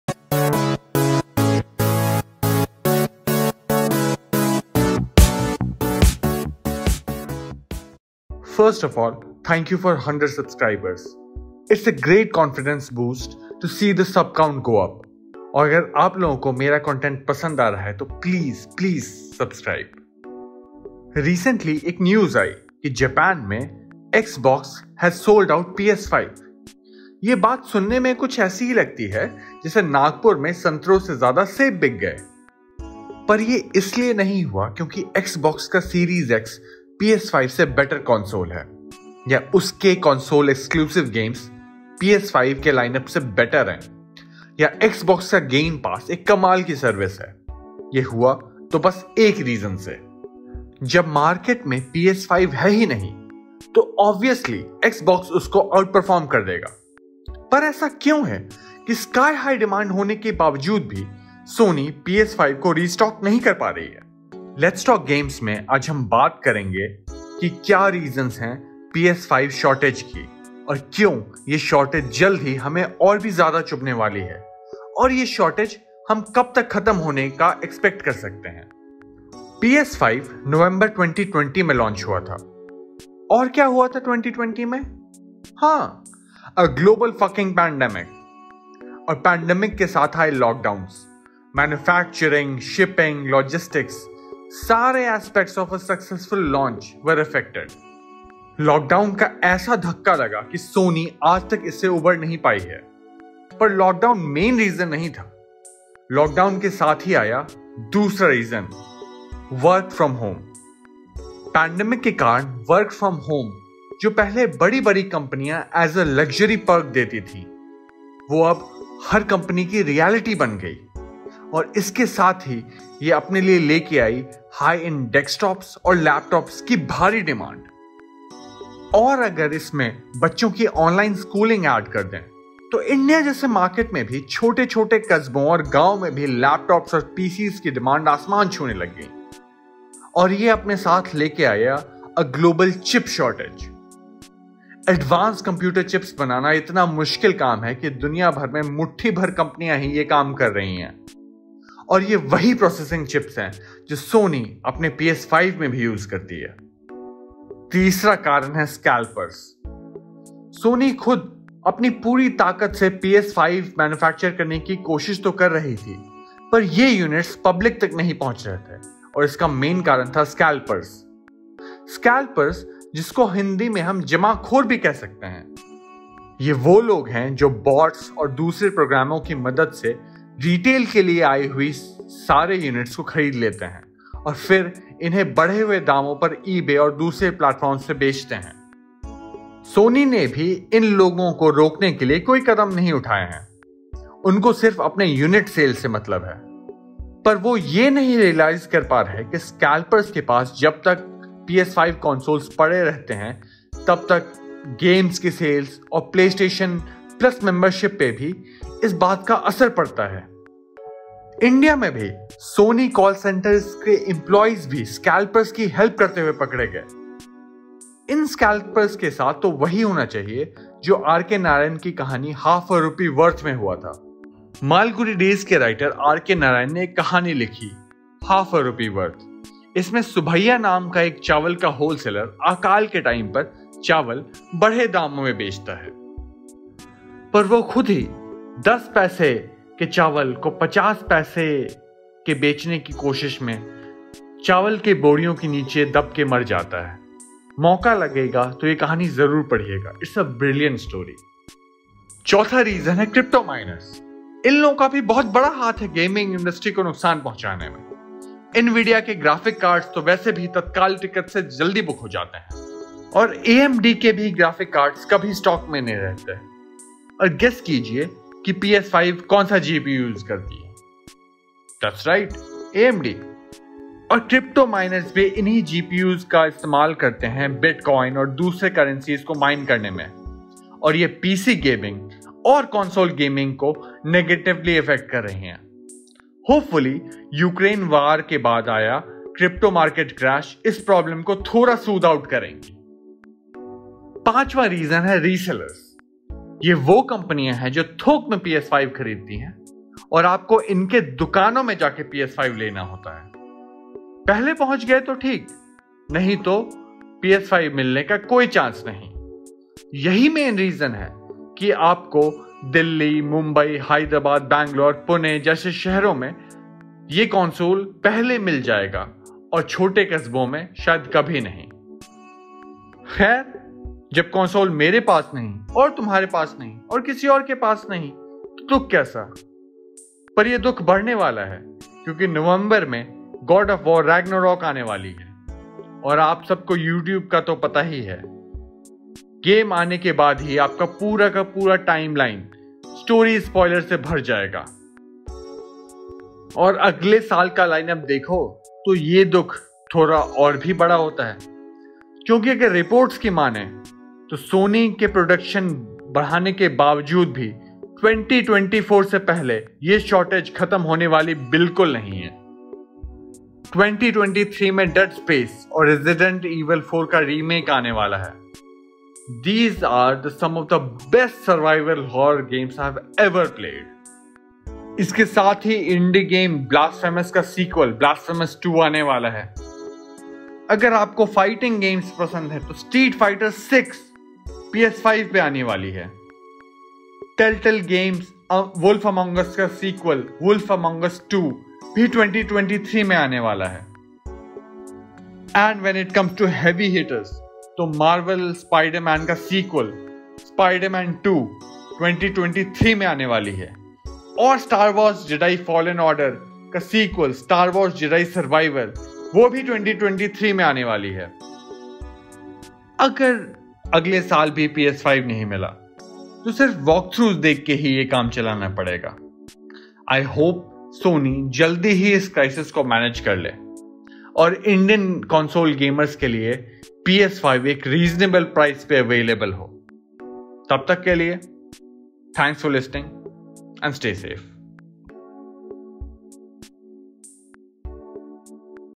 Why there is a PS5 Shortage & When will it END?-Hindi Video Essay
After the PS5’s release in 2020 there is still a worldwide shortage of the console￼. In this video we will examine the reasons behind this shortage and when can we expect the shortage to be over￼.